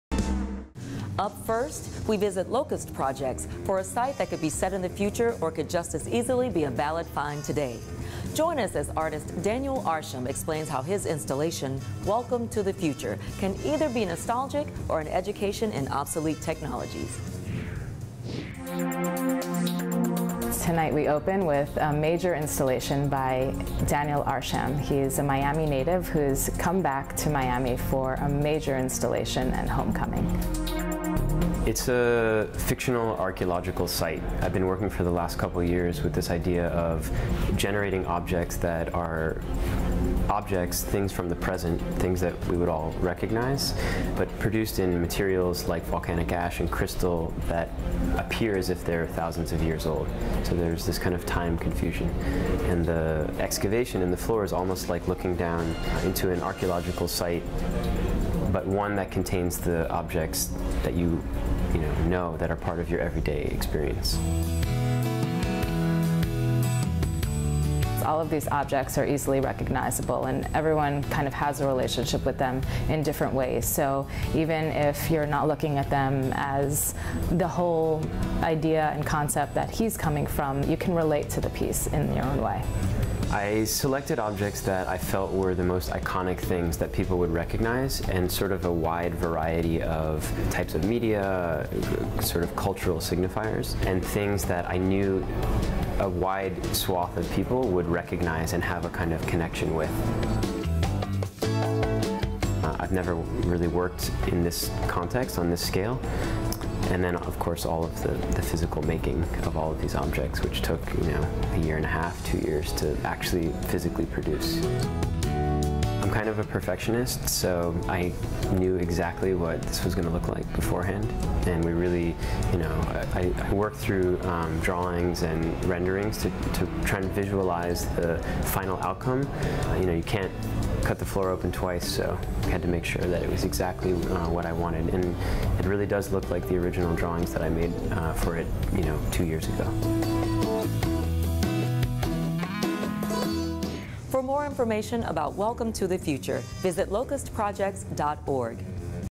Daniel Arsham | Art Loft 317 Segment
Daniel Arsham's installation, Welcome To The Future, at Locust Projects, in Wynwood, can be either nostalgic or educational in obsolete technologies. You decide.